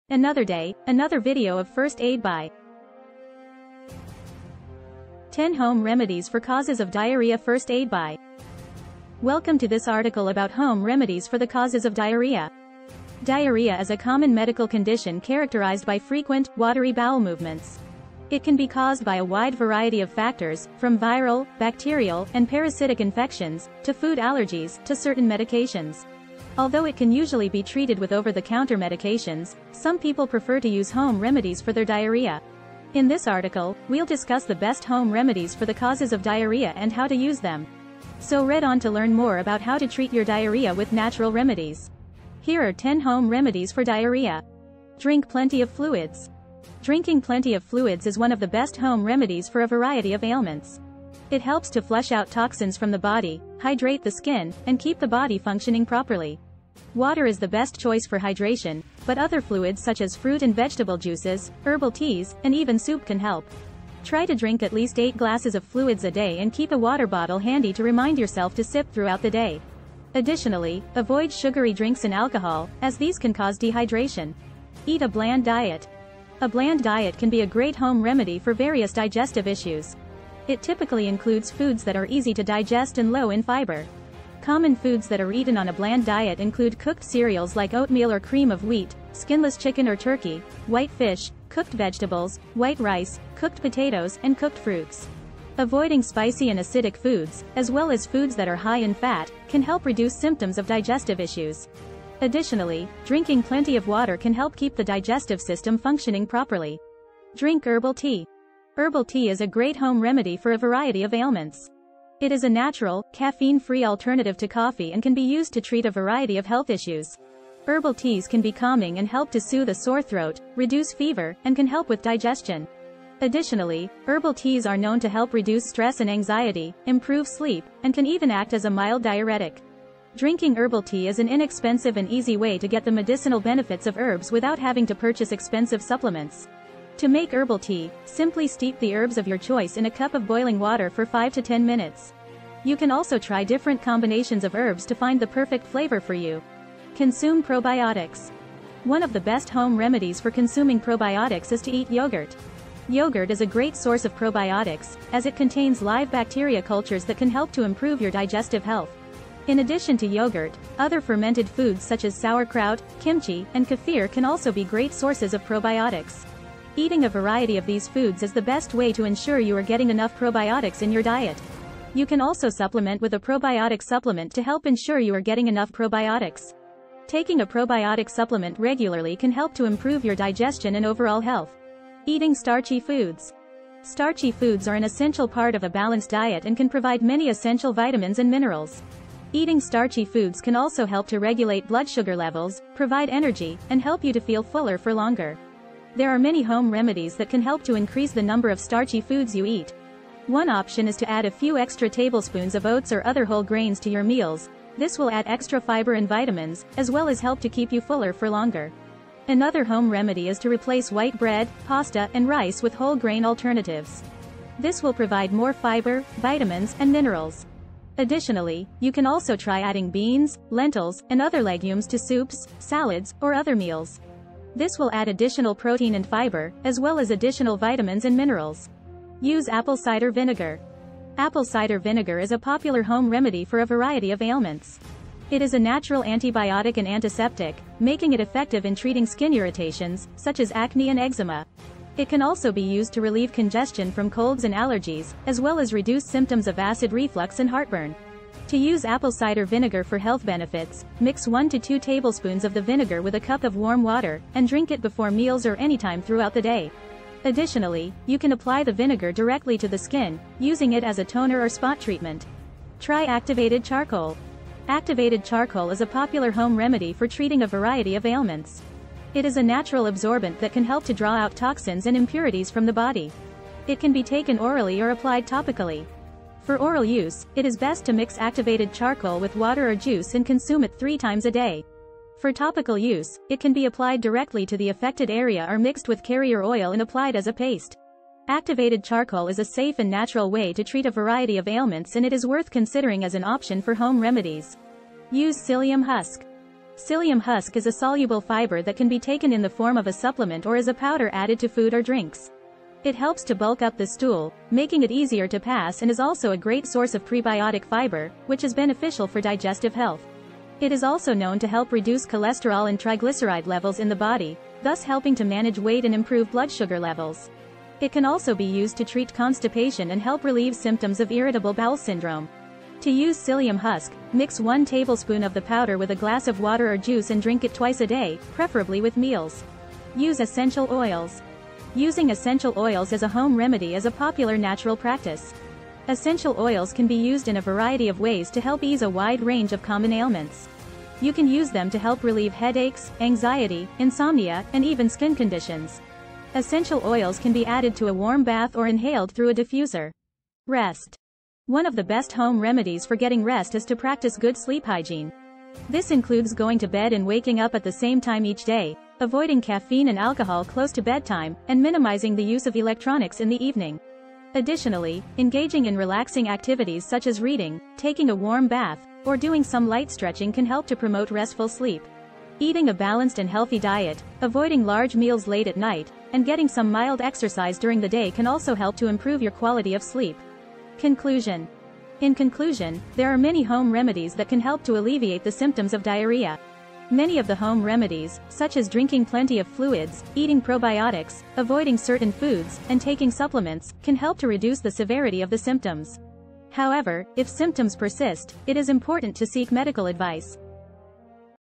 10 Home Remedies For causes of diarrhea First Aid Buy
10 Home Remedies For causes of diarrhea   First Aid Buy

Are you experiencing diarrhea and looking for natural ways to ease your symptoms? Diarrhea is a common problem that can be caused by a variety of factors such as viruses, bacteria, food intolerance, or stress. In this video, we will discuss 10 home remedies that can help you relieve the symptoms of diarrhea and promote faster recovery.

First on our list is ginger, a natural anti-inflammatory that can help soothe the digestive tract and reduce inflammation. We will also discuss probiotics, which are beneficial bacteria that can restore balance to the gut microbiome and improve digestion. Another effective remedy is apple cider vinegar, which can help kill harmful bacteria in the gut and promote a healthy environment for beneficial bacteria.

We will also cover the benefits of peppermint, which can reduce cramps and improve digestion, and chamomile tea, which has anti-inflammatory and anti-spasmodic properties that can ease diarrhea symptoms. Additionally, we will discuss the benefits of turmeric, which has antibacterial and anti-inflammatory properties, and bananas, which are rich in fiber and can help absorb excess water in the gut.

Other remedies we will discuss include drinking plenty of fluids to prevent dehydration, consuming bone broth to provide essential nutrients, and avoiding foods that can irritate the gut such as spicy or fatty foods.

If you are experiencing diarrhea, these natural remedies can help you alleviate your symptoms and promote faster recovery. However, if your symptoms persist or worsen, it is important to consult with a healthcare provider. So, watch the video and learn more about these effective home remedies for diarrhea!